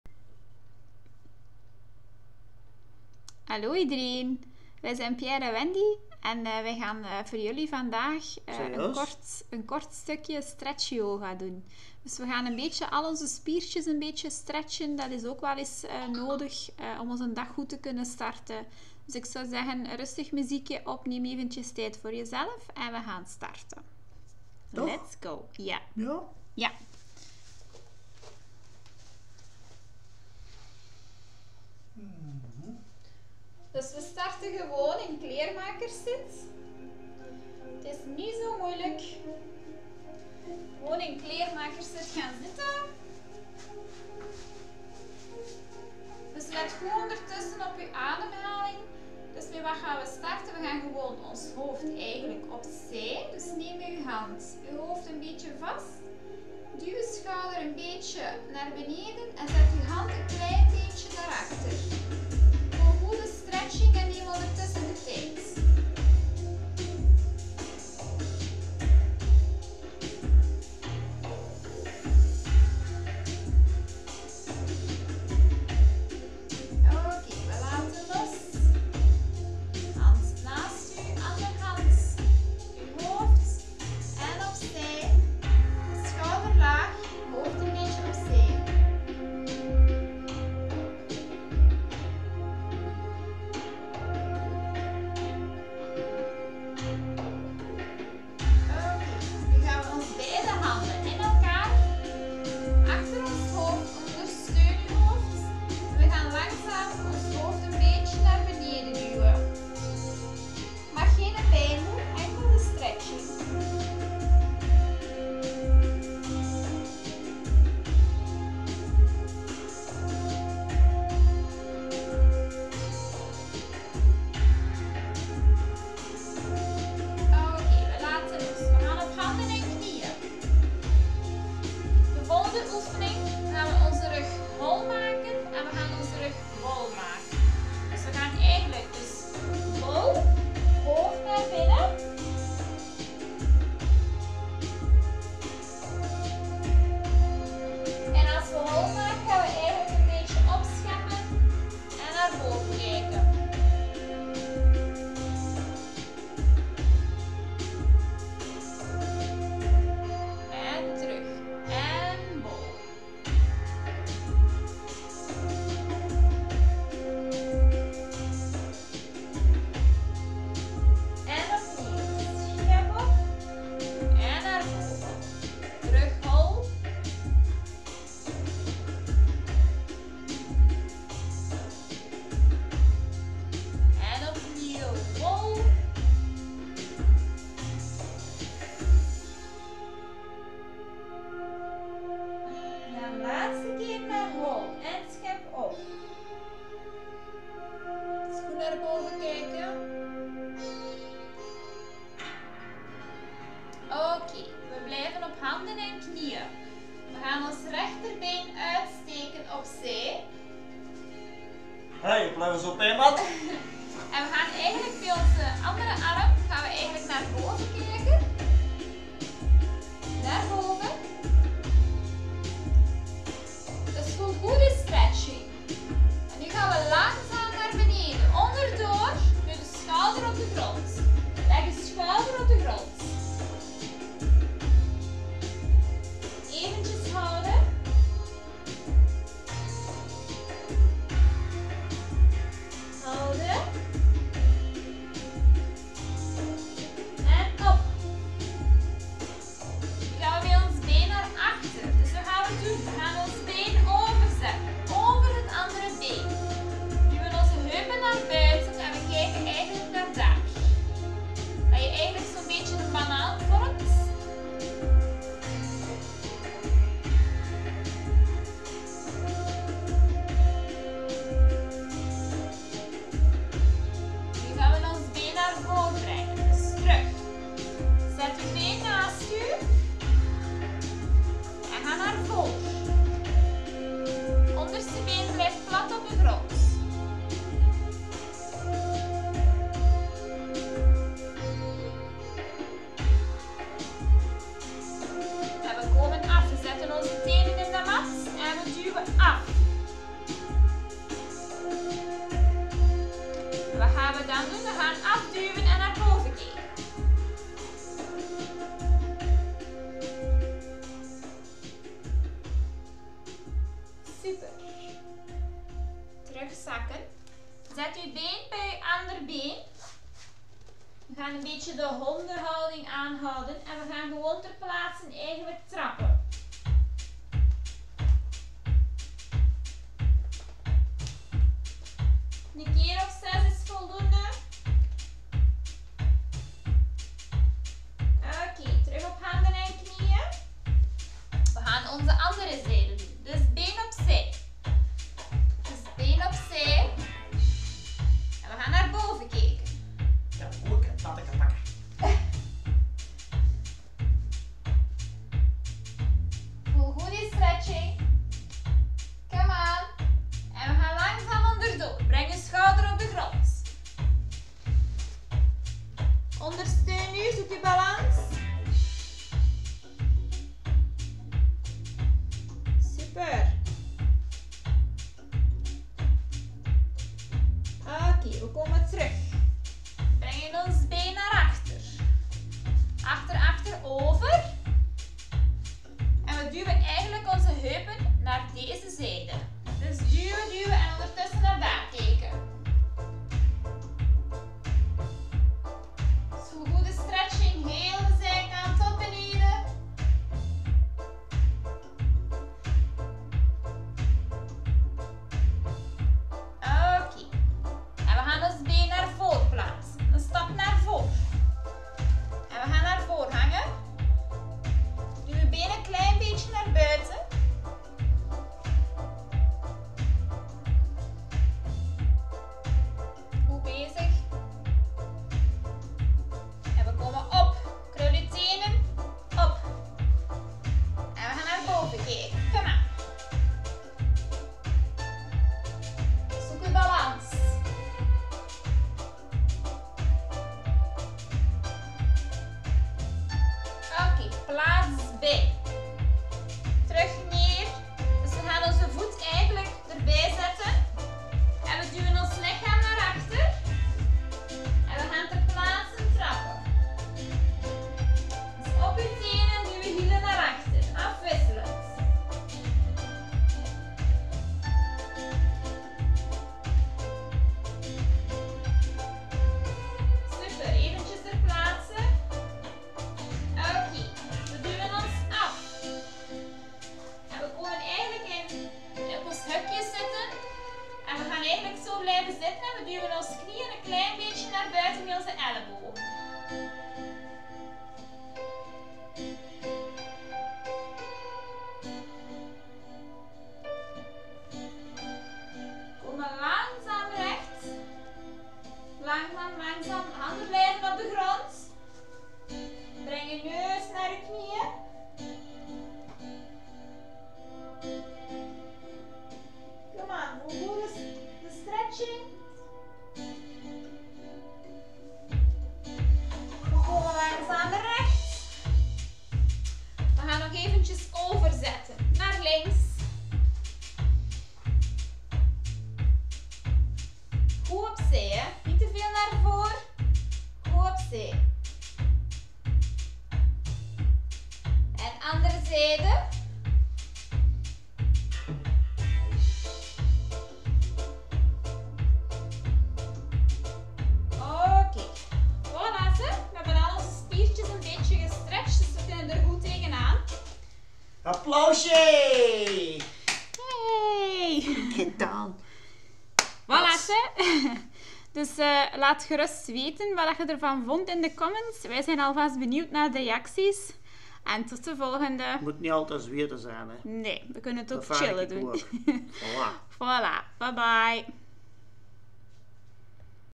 Stretch yoga.
Workoutje dat je snel kan doen om je dag goed te starten.